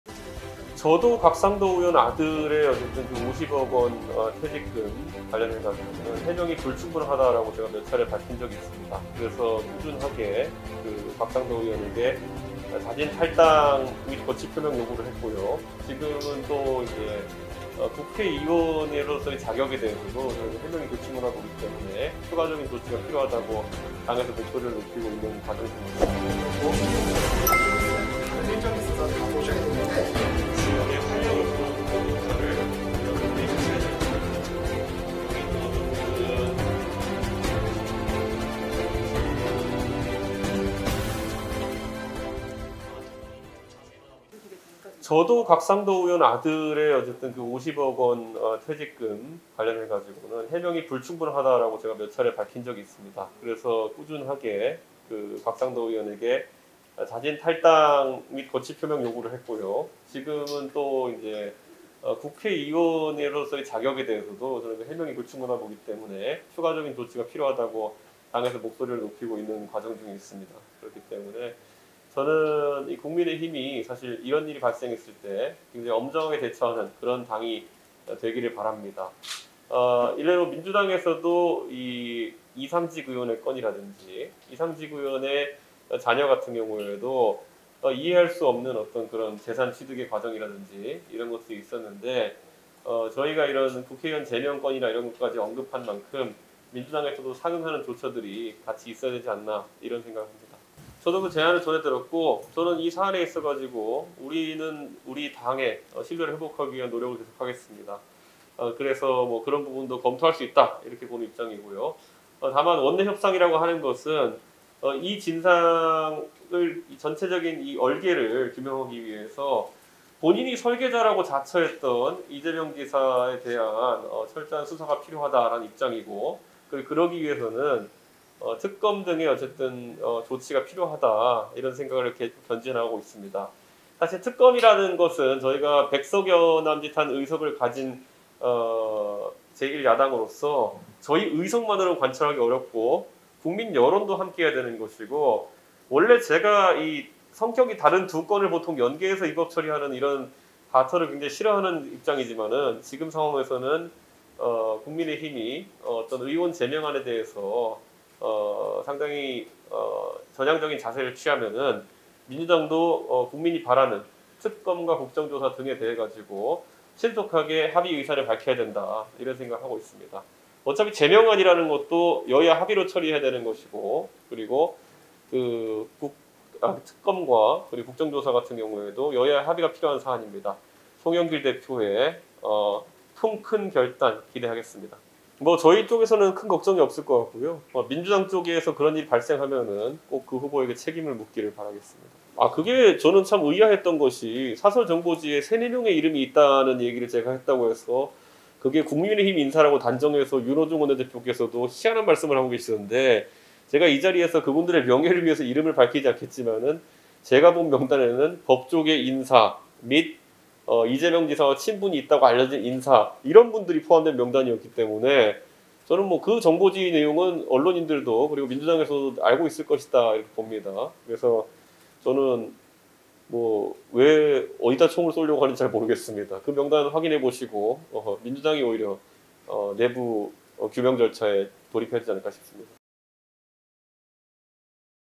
이준석 "곽상도, 의원 자격에 대한 해명 불충분…제명안 처리 검토" | 영남일보
국민의힘 이준석 대표가 29일 아들의 '화천대유 50억원 퇴직금' 논란으로 탈당한 곽상도(대구 중구-남구) 의원에 대해 "국회의원으로서의 자격에 대해서도 해명이 불충분하다고 보기 때문에 추가적인 조치가 필요하다"고 말했다. 국회 차원의 의원직 제명을 거듭 언급한 셈이다.

이 대표는 경북 경산에서 열린 국민의힘 영남캠퍼스 총회를 앞두고 기자들과 만나 "국민의힘이 이런 일이 발생했을 때 굉장히 엄정하게 대처하는 당이 되기를 바란다"며 이같이 말했다. 그러면서 "저희가 국회의원의 제명까지 언급한 만큼 더불어민주당에서도 상응하는 조처가 있어야 한다"고 강조했다.

대구·경북지역 종합일간지 '영남일보'